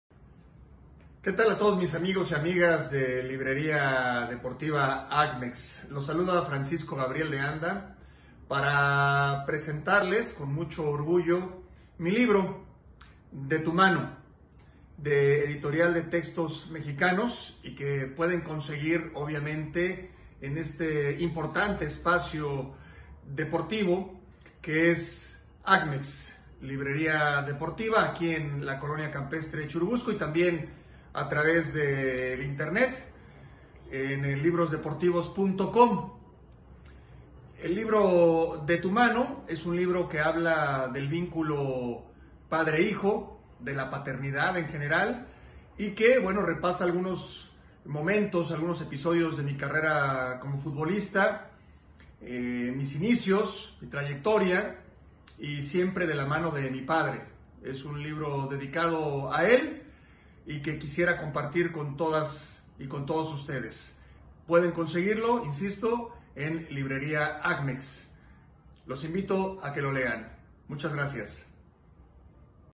José Francisco Gabriel de Anda presenta su libro "De tu mano"
José Francisco Gabriel de Anda presenta su libro "De tu mano" de venta en Librería Deportiva AGMEX y